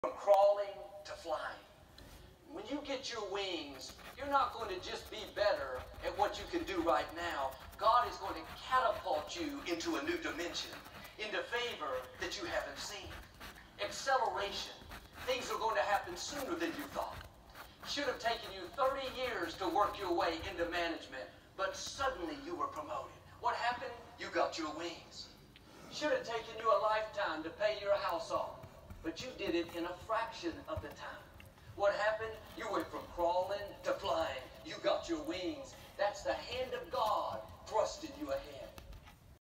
Painting Jerry Garcia
Painting Jerry Garcia, and listening to Joel Olsteen.